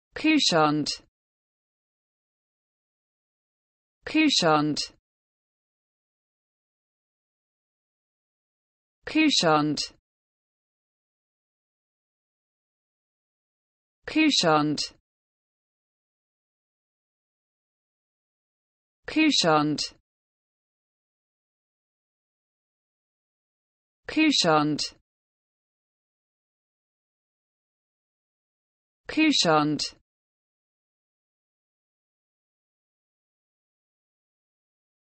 the correct pronunciation of couchant in English.
This video shows you how to pronunciation couchant in English.